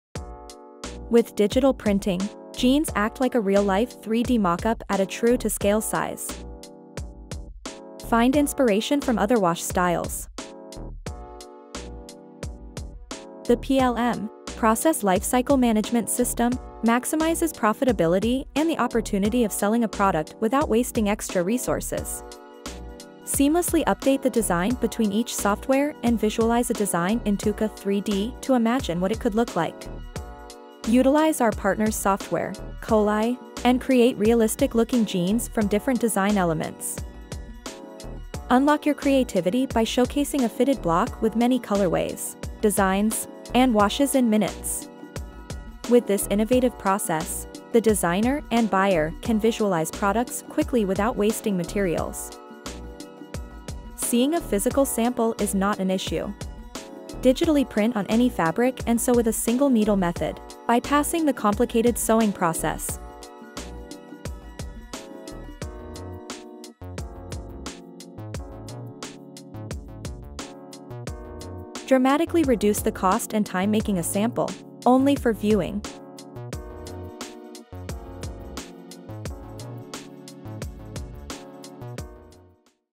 Visualize Jeans as a Real Life 3D Mockup | Digital Pigment Transfer
We created realistic looking jeans with multiple washes and colorways in TUKA3D and KolAi. The jeans were digitally printed on white bull denim which saves water, chemicals, and time to make a viewing sample so that designers or buyers can make decisions without wasting physical resources.